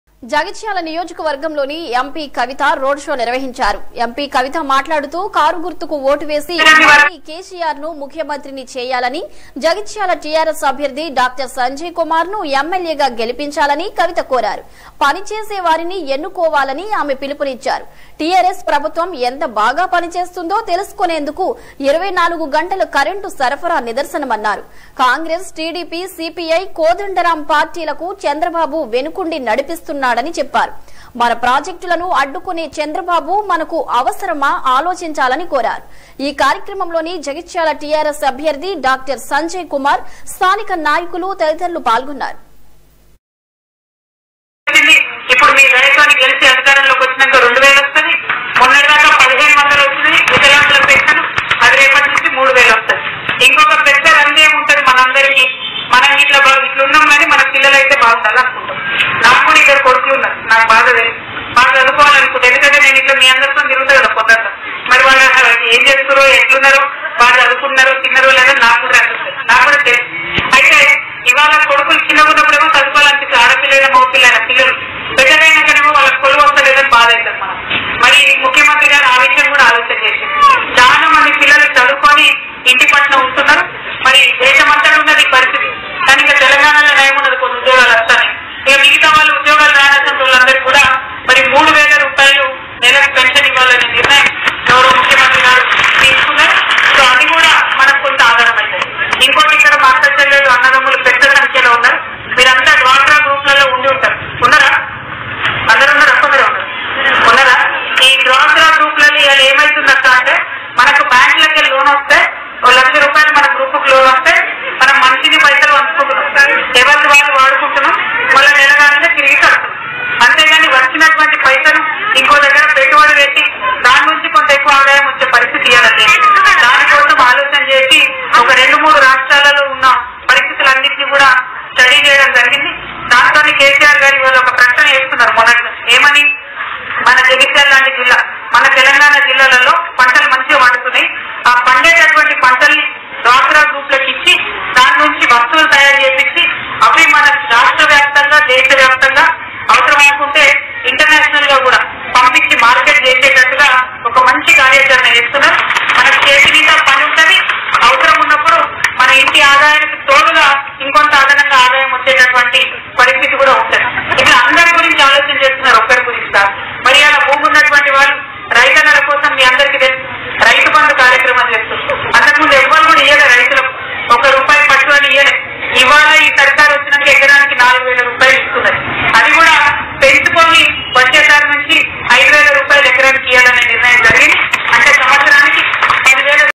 జగిత్యాల నియోజక వర్గంలో ఎంపీకవిత రోడ్ షో
జగిత్యాల నియోజక వర్గంలో ఎంపీకవిత రోడ్ షో నిర్వహించారు.  ఎంపి కవిత మాట్లాడుతూ. కారు గుర్తుకు ఓటు వేసి మల్లి కేసీఆర్ ను ముఖ్యమంత్రినీ చేయాలని, జగిత్యాల టిఆర్ఎస్ అభ్యర్థి డాక్టర్ సంజయ్ కుమార్ ను ఎమ్మెల్యేగా గెలిపించాలని కవిత కోరారు పని చేసేవారిని ఎన్నుకోవాలని ఆమె పిలుపునిచ్చారు